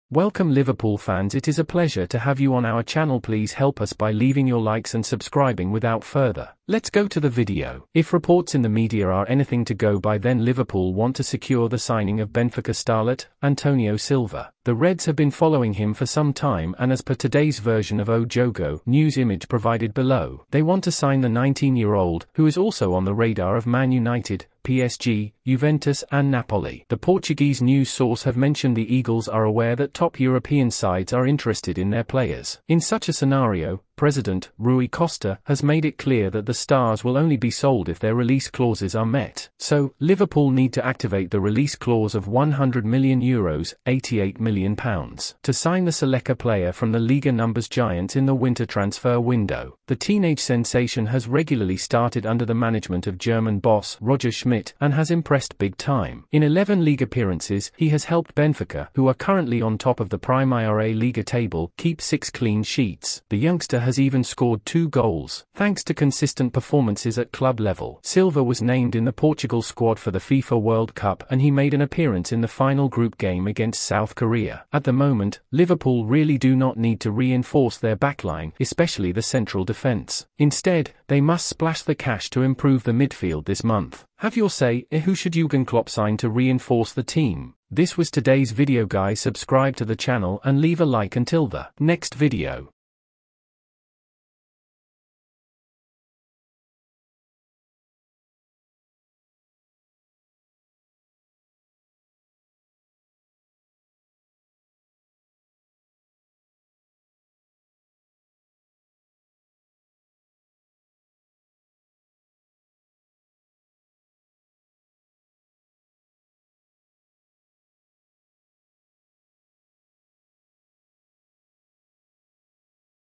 FINALLY ! THE NEWS EVERYONE WAITED FOR SKY SPORTS ANNOUNCED TODAY,LIVERPOOL TRANSFER NEWS
If reports in the media are anything to go by then Liverpool want to secure the signing of Benfica starlet, Antonio Silva.
The Reds have been following him for some time and as per today’s version of O Jogo (news image provided below), they want to sign the 19-year-old, who is also on the radar of Man Utd, PSG, Juventus and Napoli.
The Portuguese news source have mentioned the Eagles are aware that top European sides are interested in their players.
In such a scenario, president, Rui Costa, has made it clear that the stars will only be sold if their release clauses are met.
So, Liverpool need to activate the release clause of 100 million euros (£88million) to sign the Seleccao player from the Liga Nos giants in the winter transfer window.
The teenage sensation has regularly started under the management of German boss, Roger Schmidt, and has impressed big time.
In 11 league appearances, he has helped Benfica, who are currently on top of the Primeira Liga table, keep 6 clean sheets. The youngster has even scored 2 goals.
Thanks to consistent performances at club level, Silva was named in the Portugal squad for the FIFA World Cup and he made an appearance in the final group game against South Korea.
At the moment, Liverpool really do not need to reinforce their backline, especially the central defense. Instead, they must splash the cash to improve the midfield this month.
Have your say – Who should Jurgen Klopp sign to reinforce the team?